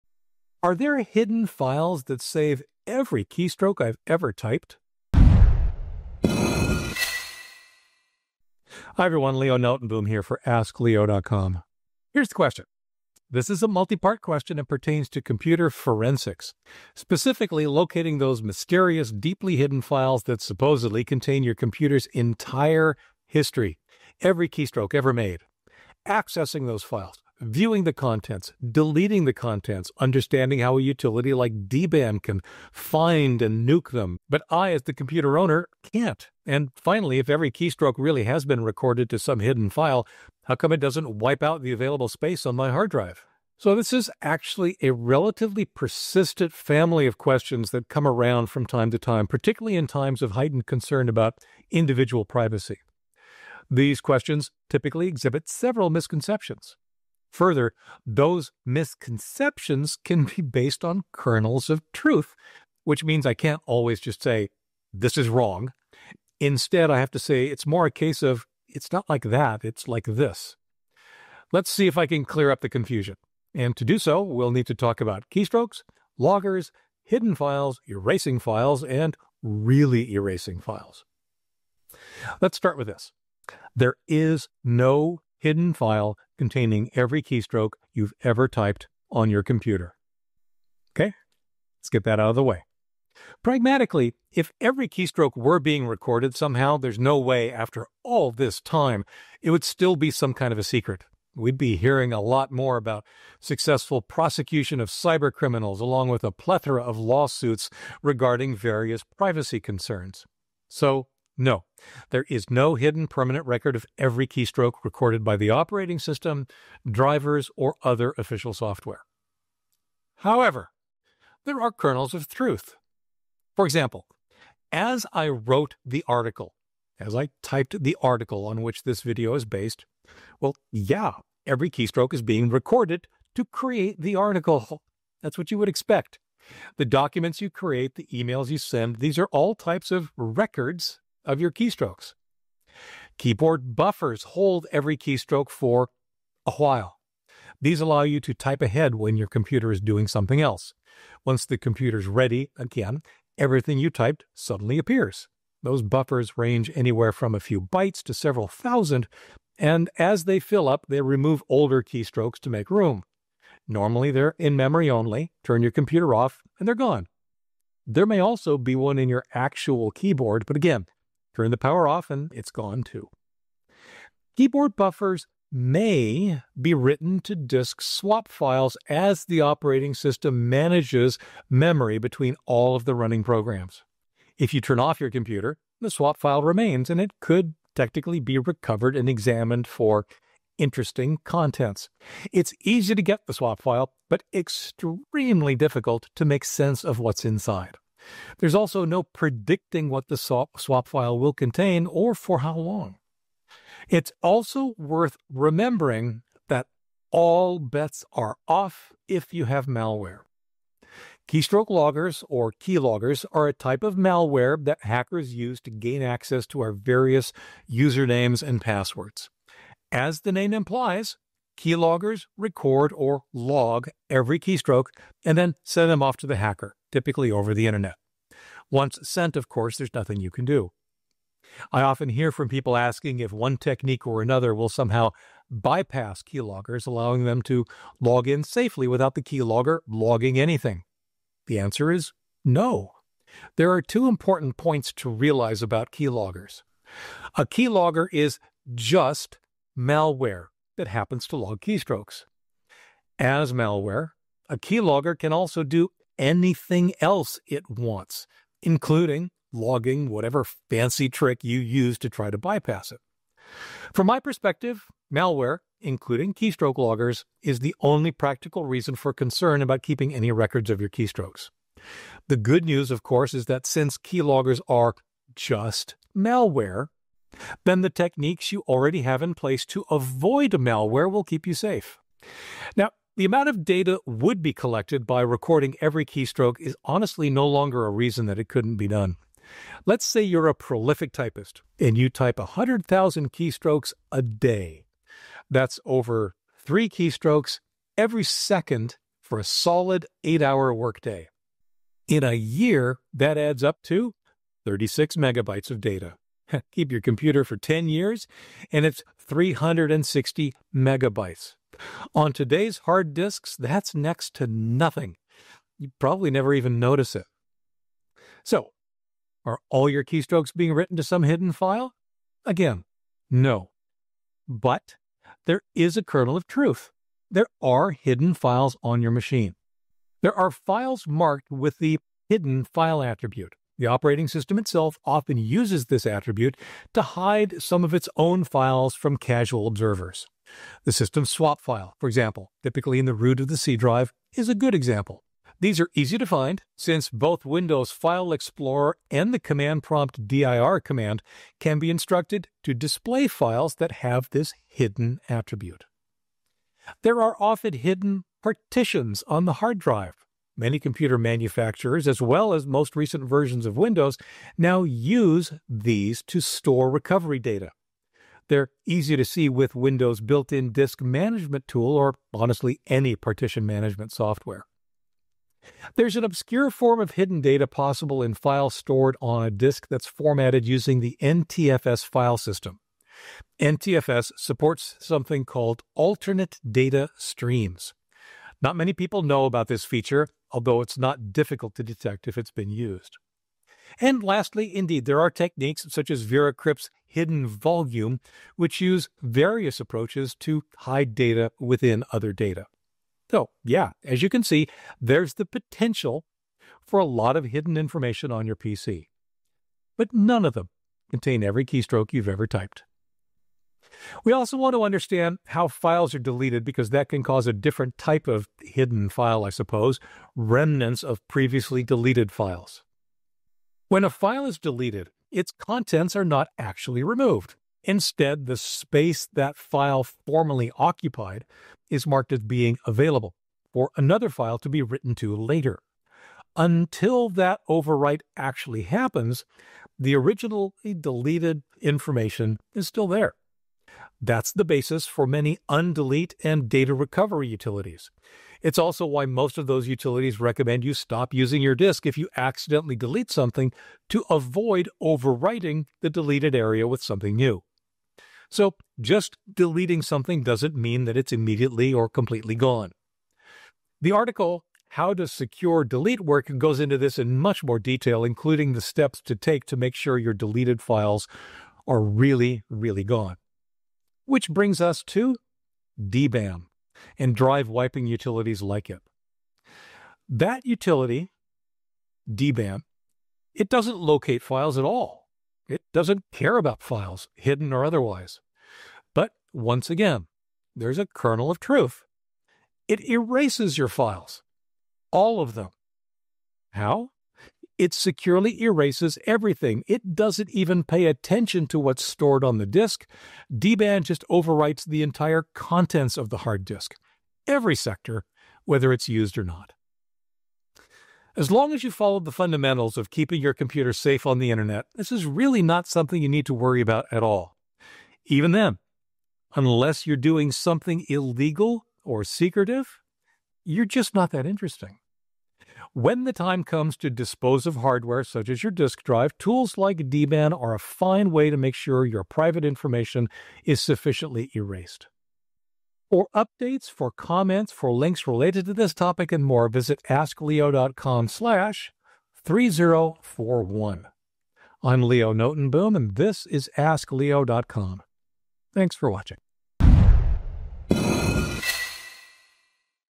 Are There Hidden Files that Save Every Keystroke I’ve Ever Typed?
⏰ Worried your computer is secretly saving everything you type? It’s not. While there are hidden files and places keystrokes can live temporarily, there’s no master file of your entire typing history (unless malware is involved). Learn what’s real, what’s myth, and how to truly erase your data.

⏰ A record of every keystroke ever?
No, your computer isn’t secretly saving every word you type. Some programs and parts of your computer remember things for a short time, but nothing keeps it all forever… unless, of course, you’ve got malware. Stay safe, and you don’t need to worry. Just erase things properly when you’re done.

Chapters
0:00 Hidden Files Saving Every Keystroke
2:00 Recording keystrokes
3:30 Keystroke loggers
5:30 Hidden files
6:50 Deleting files
7:55 Drive-wiping utilities